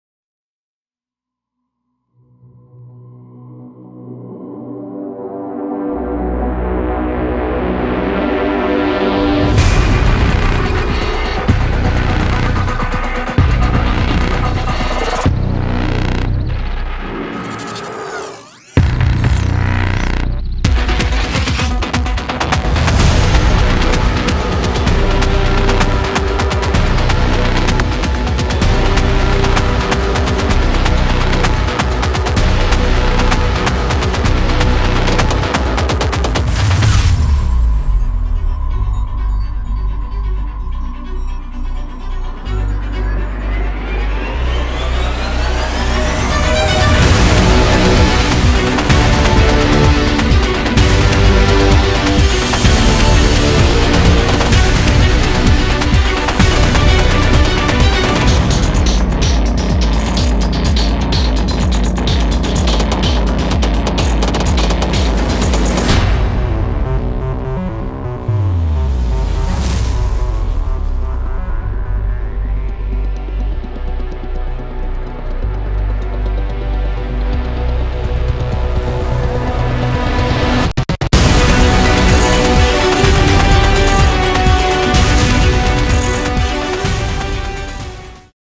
Piet Hein 2016 Fall [Intro]
Video intro for Providence Christian College's annual Piet Hein Awards (2016 Fall Edition).